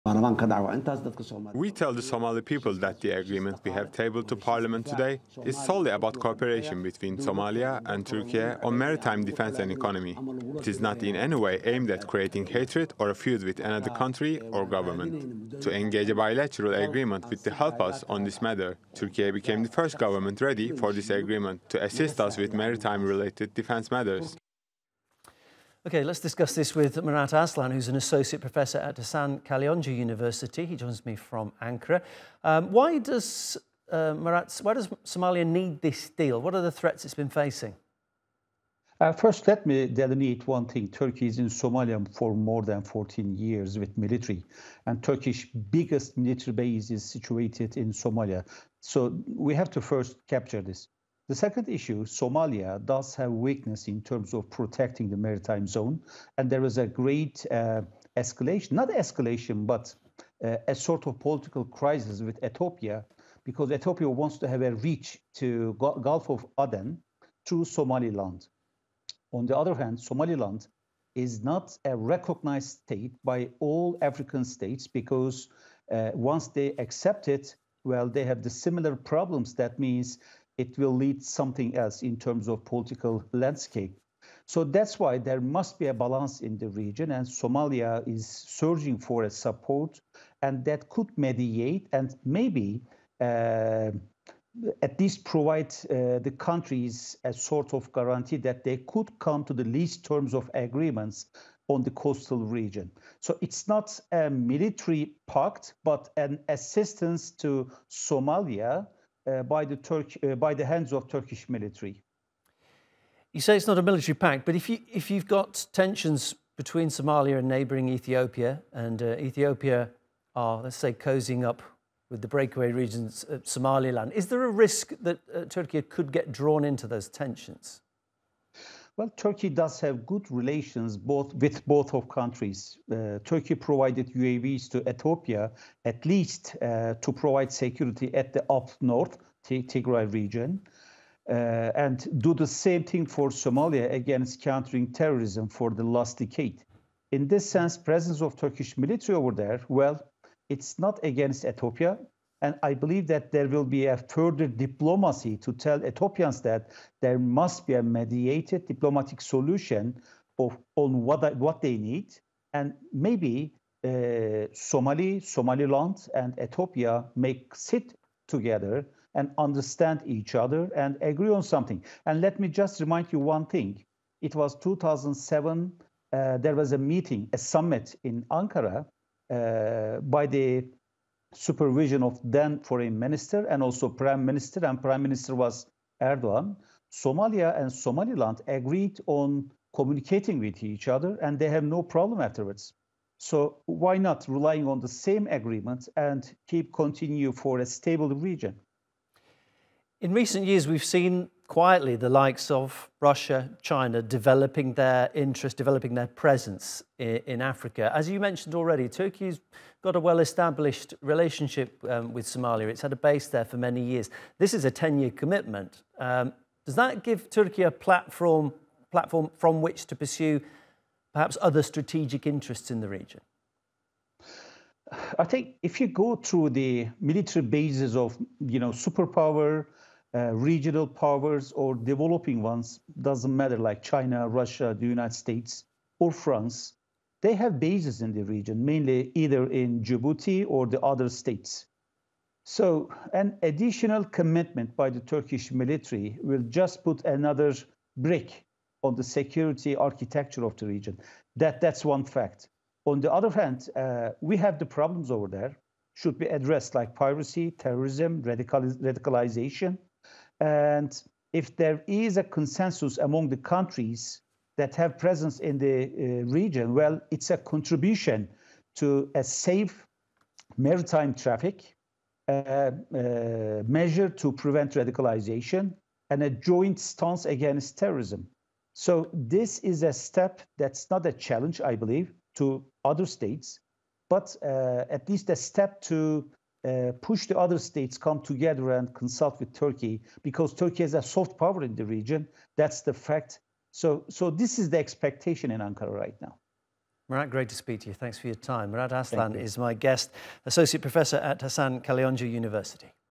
Why does Somalia need a defence pact with NATO ally Türkiye?
Somalia's parliament has ratified a defense and economic pact with Turkiye. Somalian President Hassan Sheikh Mohamud said the agreement means Turkiye will protect Somali territorial waters for the next 10 years. Murat Aslan from Hasan Kalyoncu University explains.